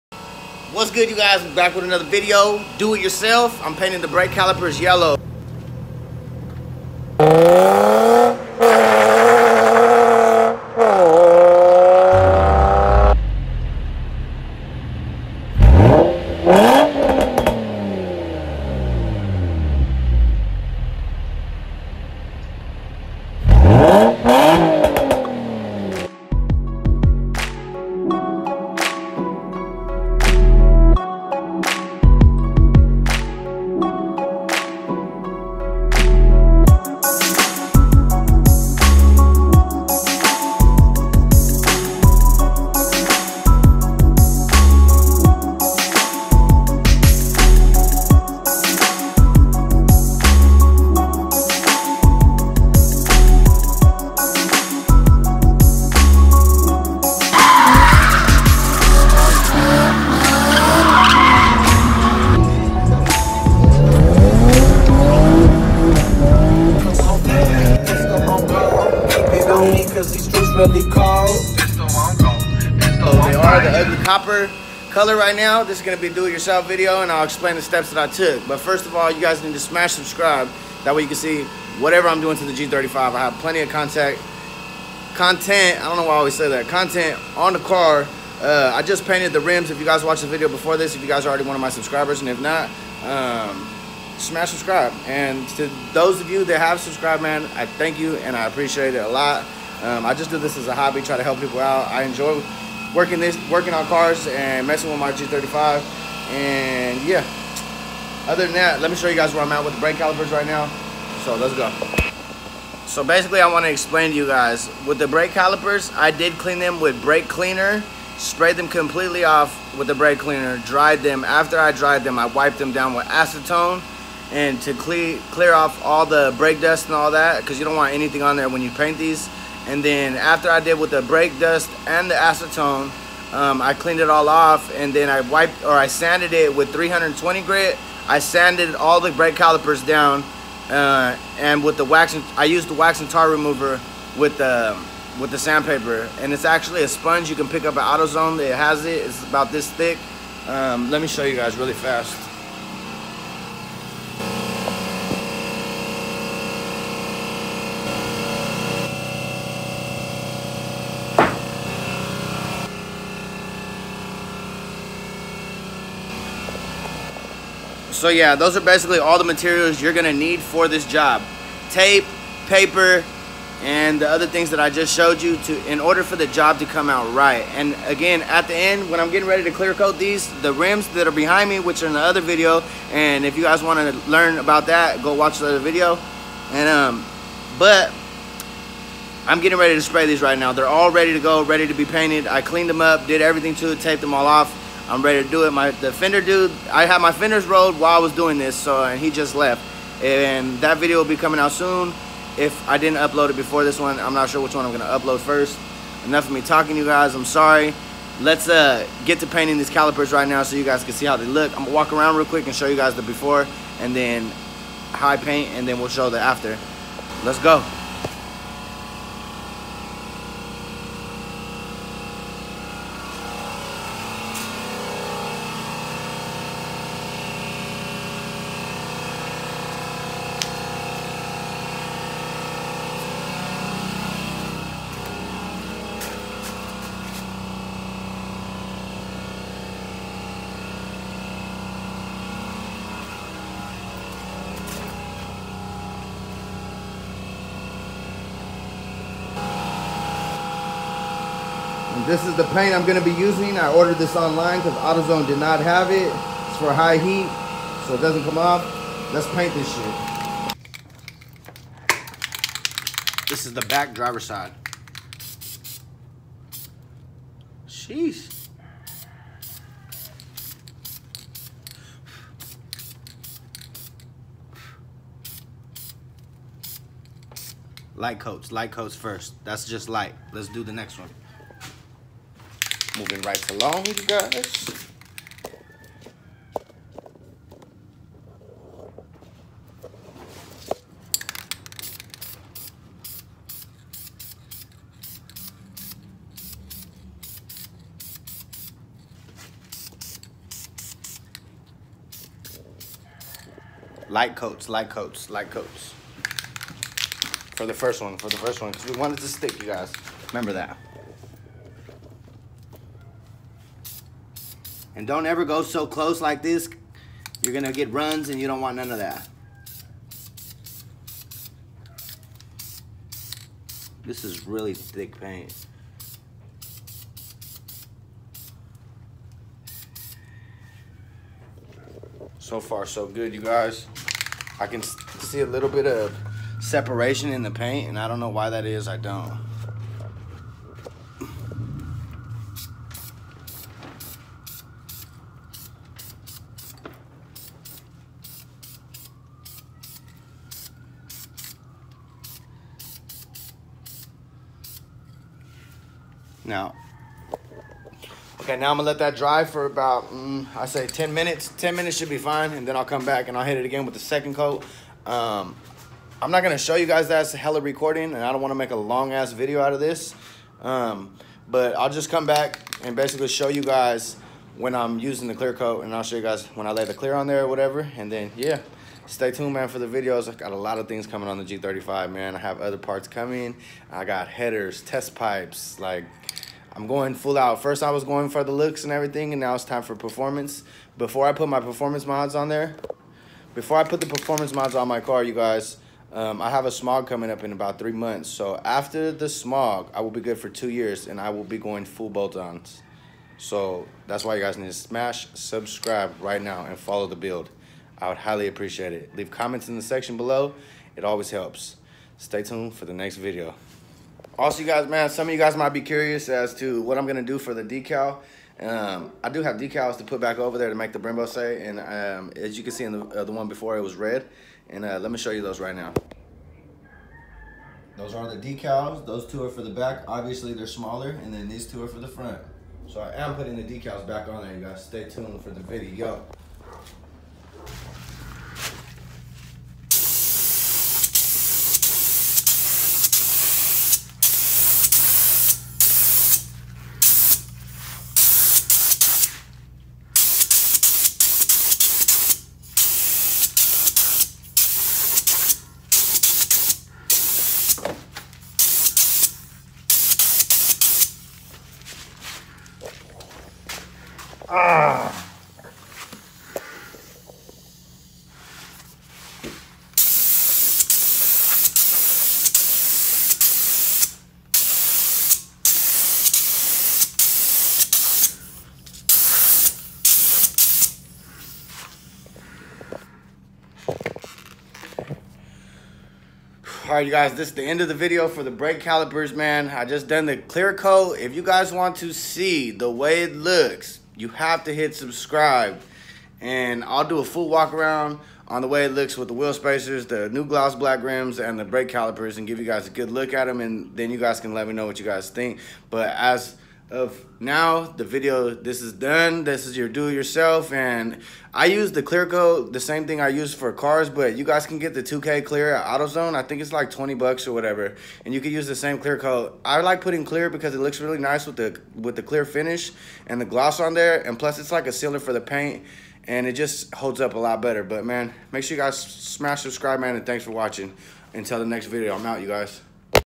DIY SPRAY PAINTING BRAKE CALIPERS (any car)
painting the calipers on my 2004 G35 bright yellow high gloss new decals .